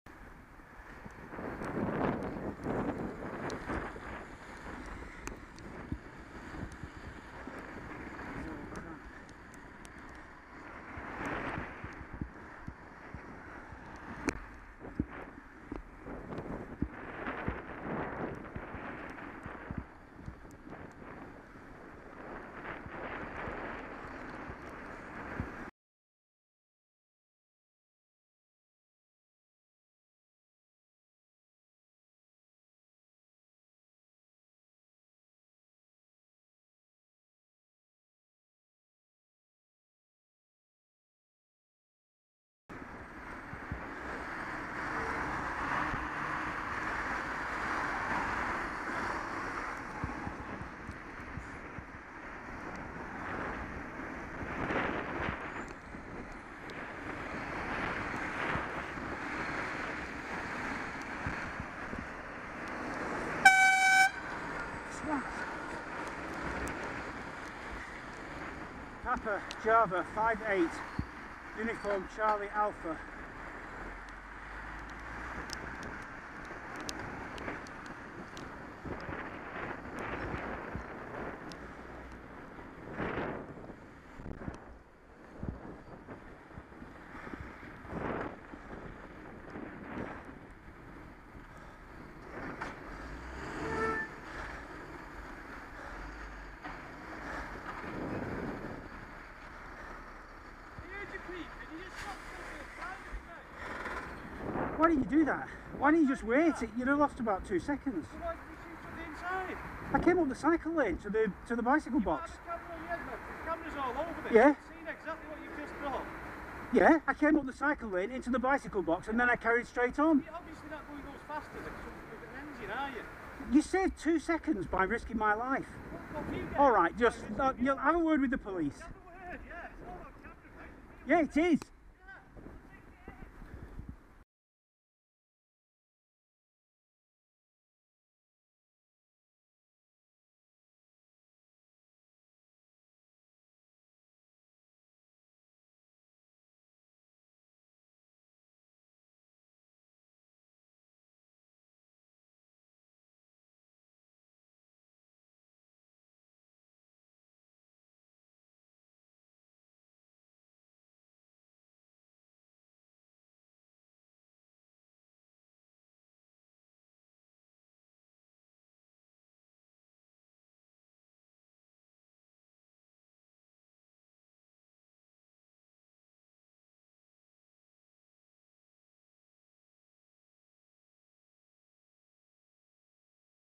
PJ58UCA - Dangerous truck driver (To: @GMPTraffic)
The discussion we had at the loading bay leads me to believe that this was a deliberate action to try to "teach me a lesson".

Location: Manchester Road, Bolton (Northbound). Junction with Trinity Street.
Date and time: 10th December 2014 at 8.50 AM.
VRN: PJ58UCA

UPDATE [11th December 2011]: The company has agreed that this driver's behaviour was not acceptable, have interviewed the driver and have given him a formal, recorded warning. I am satisfied with this outcome. The main thing is that the driver (as well as others) is left in no doubt that it is unacceptable to put vulnerable road users in danger